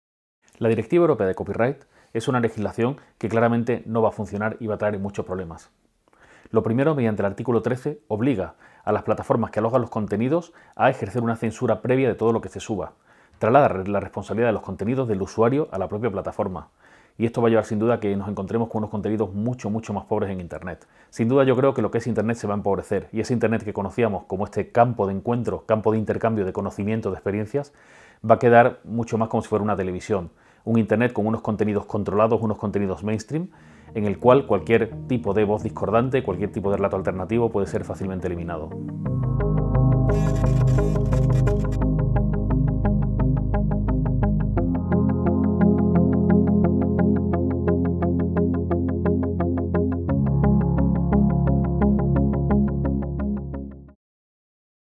Stéphane M Grueso
Cineasta.
Miembro de la Junta Directiva de la PDLI

“Con la directiva, ese internet que conocimos como campo de encuentro y de intercambio de conocimiento va a quedar para contenidos mainstream y no habrá sitio para contenidos alternativos”

Este vídeo forma parte de la campaña StopCensuraCopyright de la PDLI

Infórmate. Actúa.